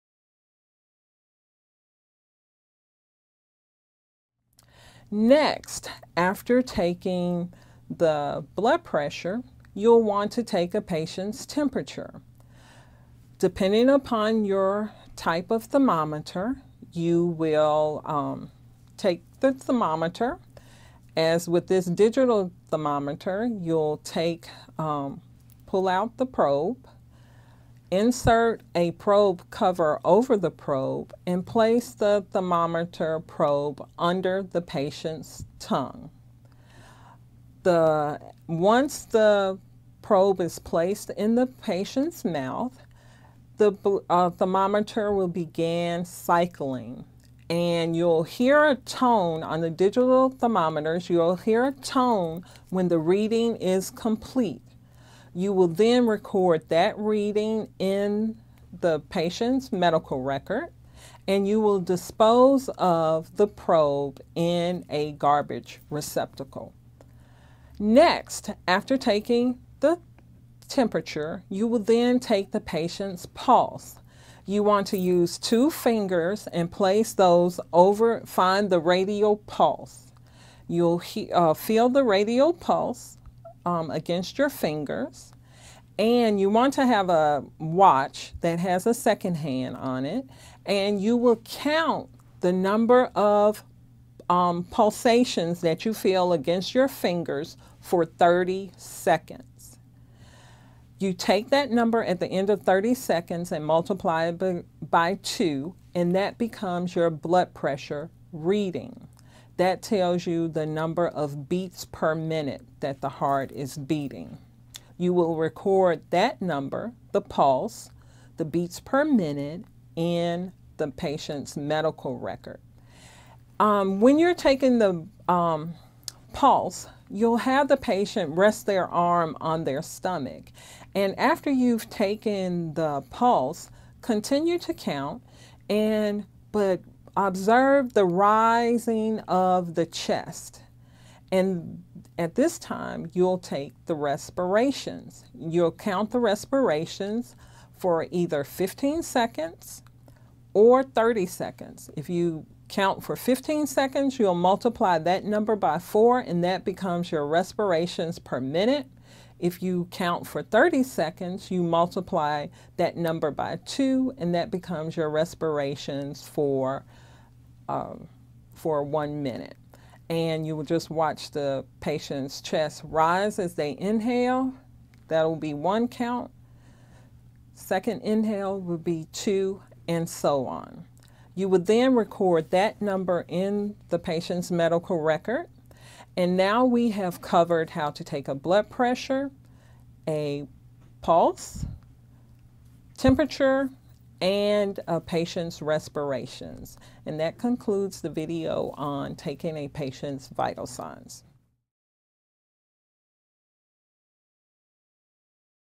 Module 5 - Taking Vital Signs Part 2
This video is the second part of a lecture on taking vital signs